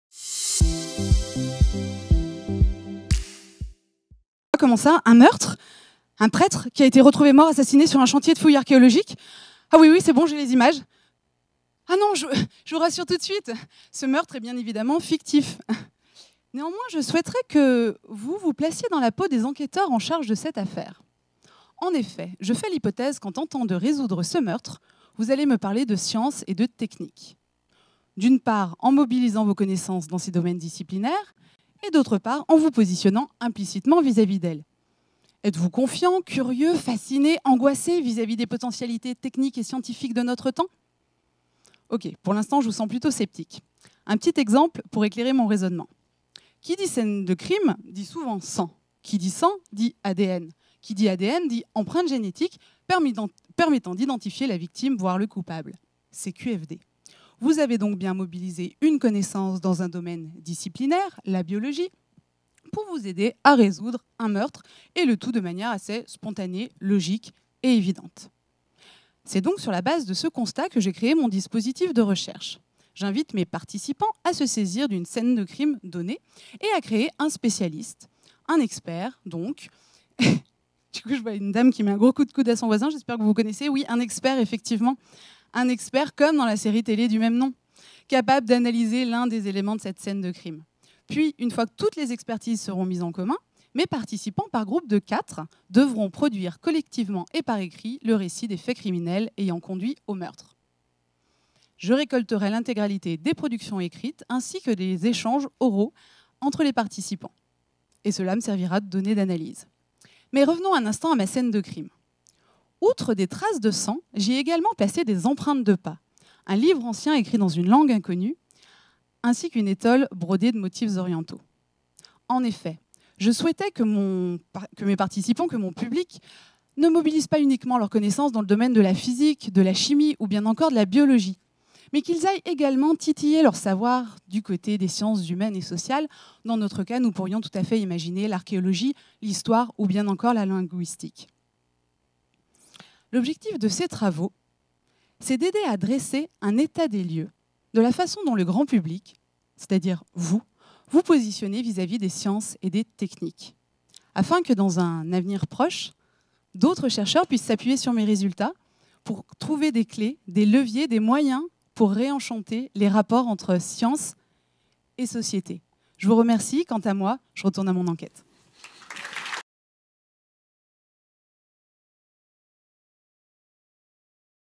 Emilie BABUIN, candidate Ma thèse en 180 secondes-Finale Université de Lyon
Emilie BABUIN a participé à la finale locale "Université de Lyon" du concours Ma thèse en 180 secondes.
La finale a eu lieu le 20 avril 2017 dans le Grand amphithéâtre de l'Université de Lyon devant près de 350 personnes.